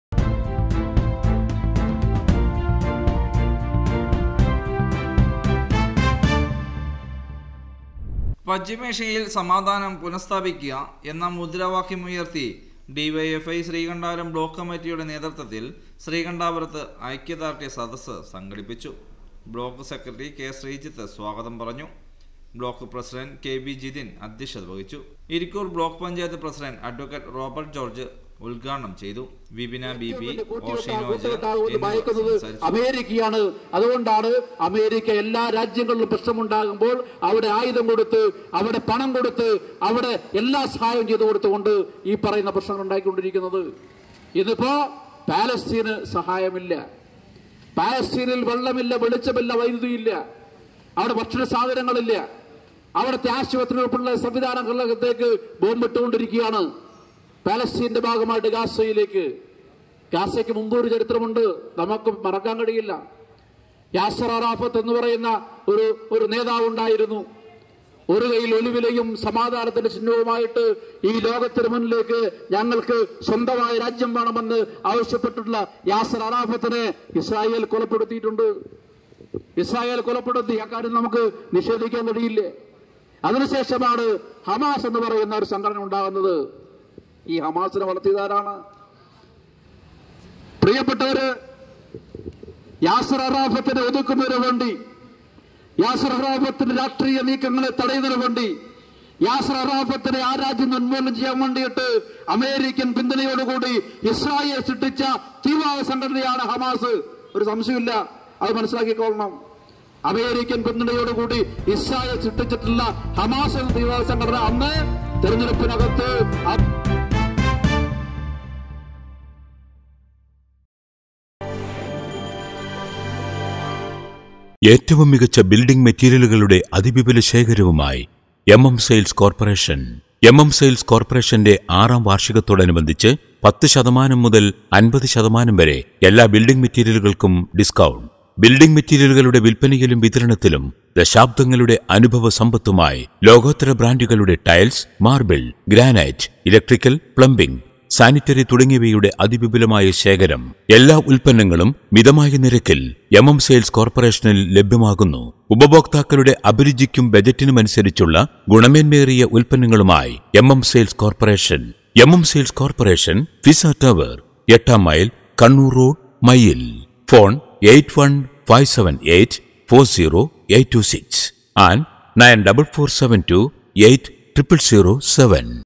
ഡിവൈഎഫ്ഐ ശ്രീകണ്ഠാപുരം ബ്ലോക്ക് കമ്മിറ്റിയുടെ നേതൃത്വത്തിൽ ശ്രീകണ്ഠപുരത്ത് ഐക്യദാർഢ്യ സദസ്സ്
പശ്ചിമേഷിയിൽ സമാധാനം പുനസ്ഥാപിക്കുക എന്ന മുദ്രാവാക്യമുയർത്തി ഡിവൈഎഫ്ഐ ശ്രീകണ്ഠാപുരം ബ്ലോക്ക് കമ്മിറ്റിയുടെ നേതൃത്വത്തിൽ ശ്രീകണ്ഠപുരത്ത് ഐക്യദാർഢ്യ സദസ്സ് സംഘടിപ്പിച്ചു. ബ്ലോക്ക് സെക്രട്ടറി കെ ശ്രീജിത്ത് സ്വാഗതം പറഞ്ഞു  ബ്ലോക്ക് പ്രസിഡന്റ് കെ വി ജിതിൻ അധ്യക്ഷനായി ഇരിക്കൂർ ബ്ലോക്ക് പഞ്ചായത്ത് പ്രസിഡന്റ് അഡ്വക്കേറ്റ് റോബർട്ട് ജോർജ് ഉദ്ഘാടനം ചെയ്തു. വിപിന ബിപി, ഒ സിനോജ് ഷിനോജ് എന്നിവർ സംസാരിച്ചു